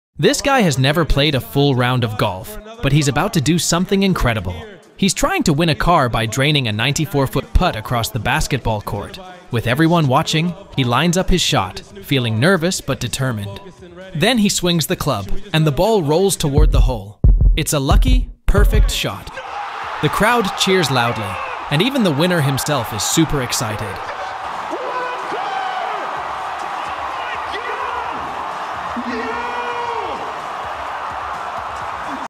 Guy Has NEVER Played Golf Before!
This guy has never played a full round of golf, but he's about to do something incredible. He's trying to win a car by draining a 94-foot putt across the basketball court. With everyone watching, he lines up his shot, feeling nervous but determined. Then he swings the club, and the ball rolls toward the hole. It's a lucky, perfect shot. The crowd cheers loudly, and even the winner himself is super excited.